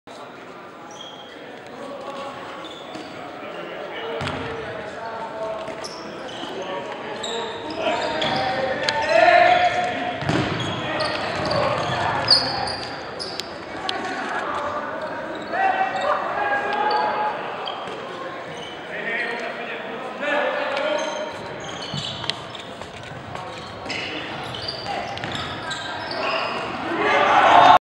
Nyíregyházi KC vs. Mezőkövesd - szon.hu
Nyíregyházi KC vs. Mezőkövesd kézilabda mérkőzés.